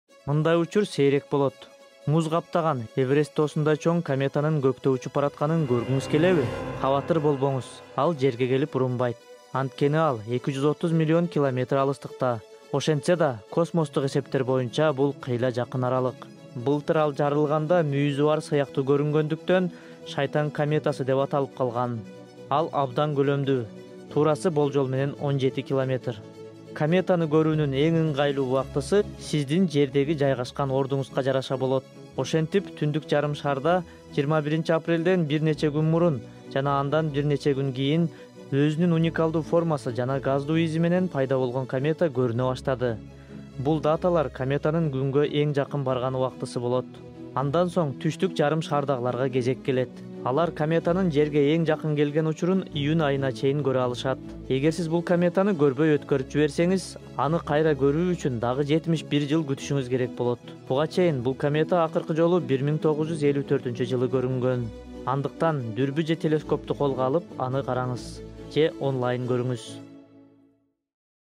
"Шайтан мүйүздүү" кометаны көргүңүз келеби? - BBC Kyrgyz
Эверест тоосундай чоң, муздуу кометанын көктө учуп бараткан сейрек учурун көргүңүз келеби? Анда дүрбү же телескопту колго алып аны көктөн караңыз же интернеттен көрүңүз.